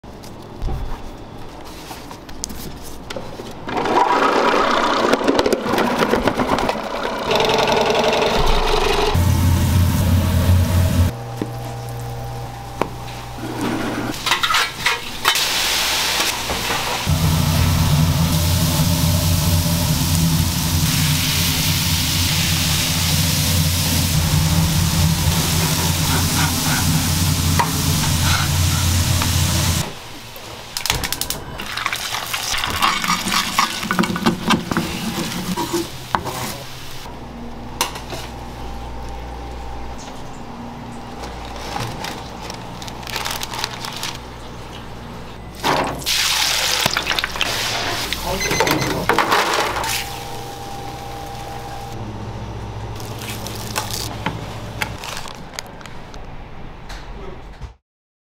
푸드트럭으로 500개씩 팔렸다는? 푸짐한 토핑 버거로 연매출 10억! 계란폭탄 버거 - American style burger / Korean street food
🧡 Information 🧡

🟤[ 가격 ]
-  스크램블폭탄 지못미버거 KRW 8,100

🟠[위치]
- 왓더버거
[Google map]

양산시 물금읍 가촌리 1263-4